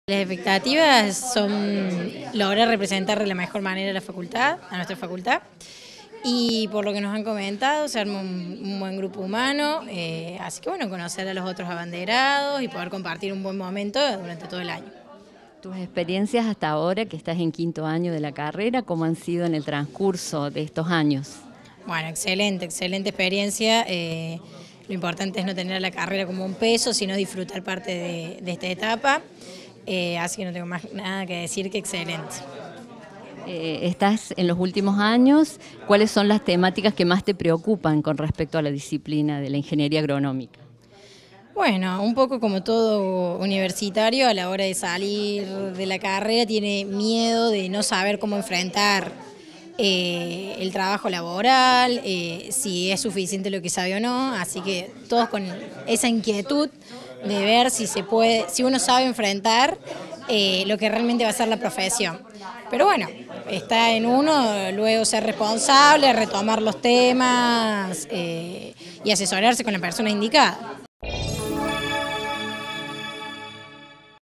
VALORÓ EL SIGNIFICADO DE HONRAR A LA FACULTAD CON EL SÍMBOLO PATRIO
FLORENCIA ALTOBELO-ABANDERADA ENTRANTE FAV
Facebook.com/uniriotv
Twitter.com/uniriotv